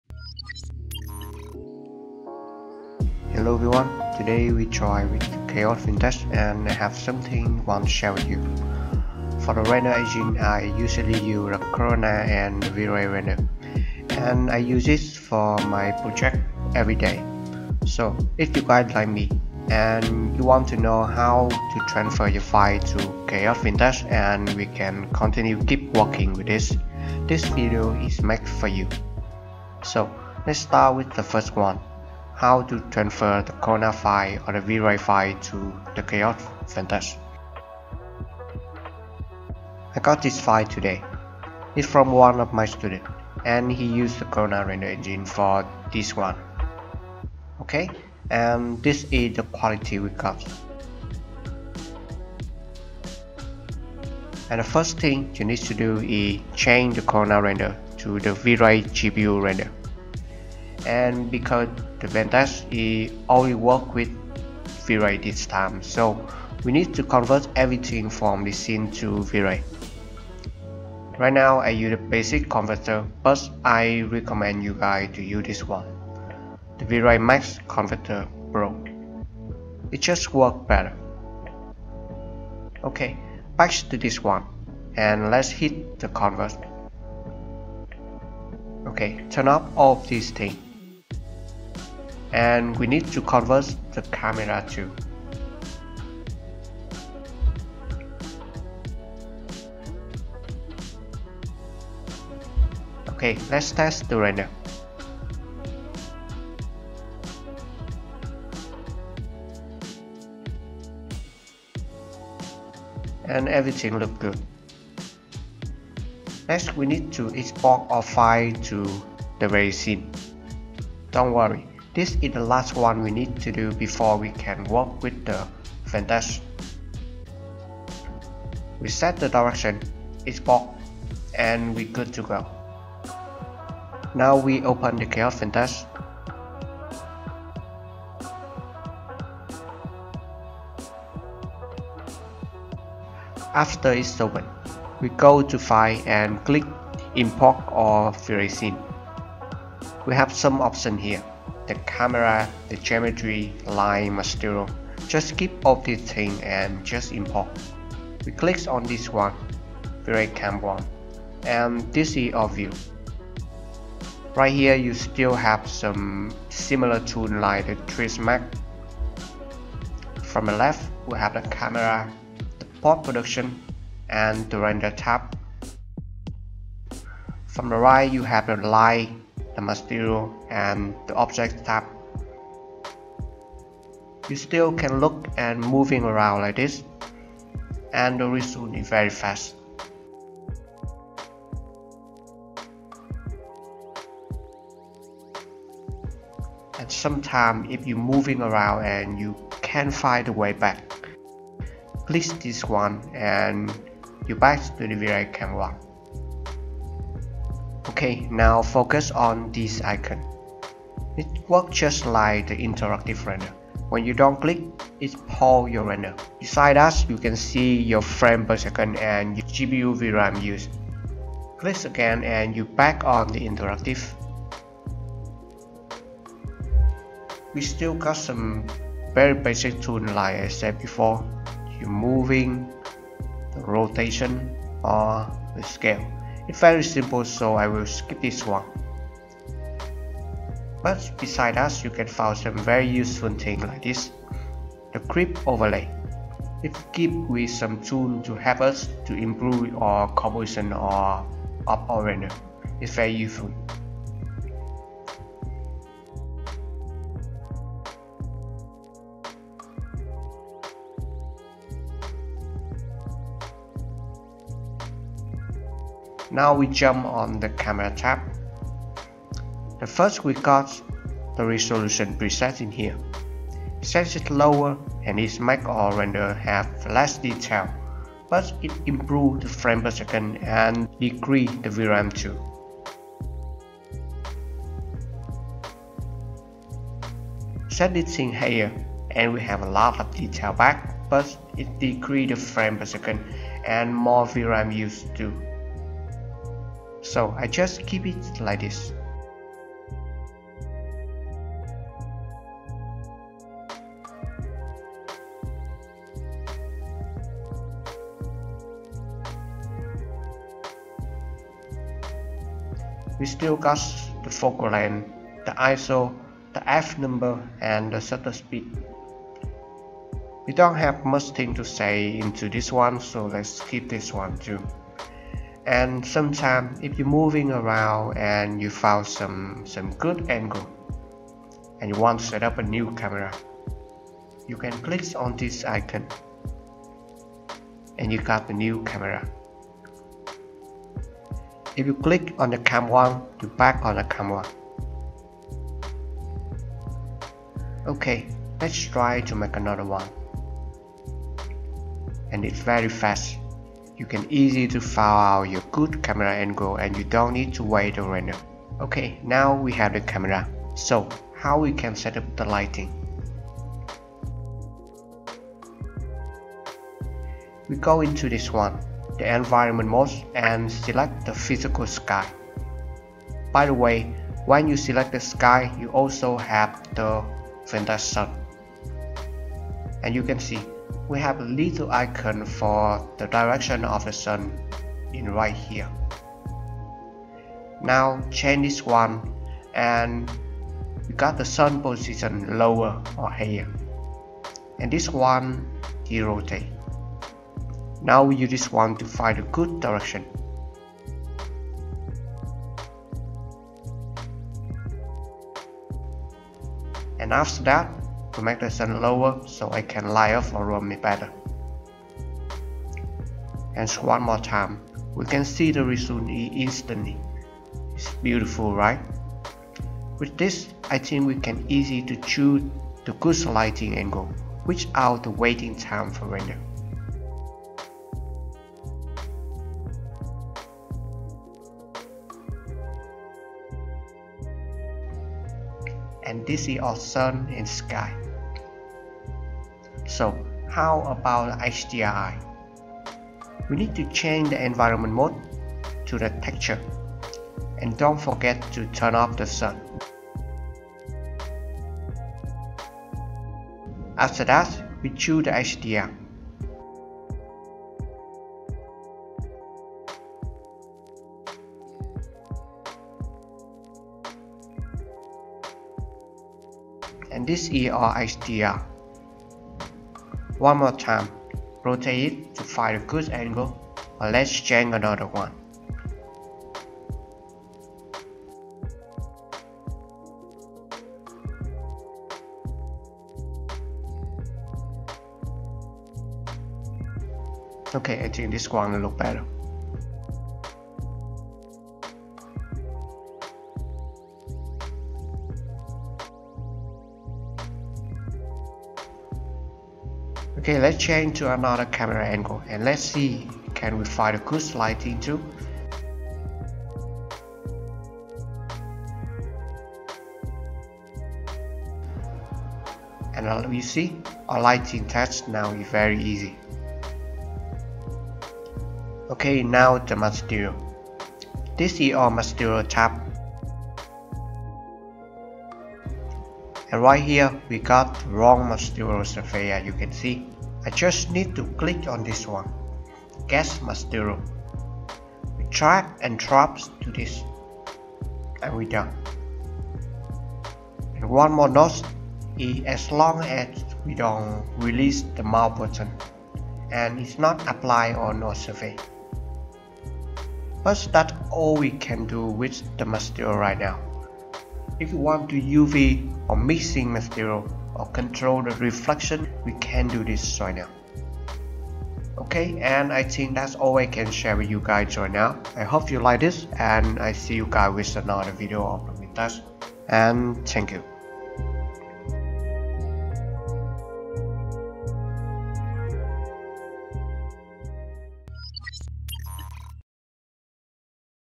Chaos Vantage Test 01 | Converting and Working with Vantage | 4Pixos Academy
Welcome to the first episode of the "Chaos Vantage Test" series 😃

In this series of articles, we will be sharing intriguing information about the remarkable features of Chaos Vantage, the latest 3D software developed by Chaos Group. If you are a Vray or Corona user, just like me, I believe you will adore this new engine.

Online Vantage English course will be starting on 24th June 2023. Register now !!
4pixos Academy - INTERNATIONAL ARCHITECTURAL VISUALIZATION TRAINING CENTER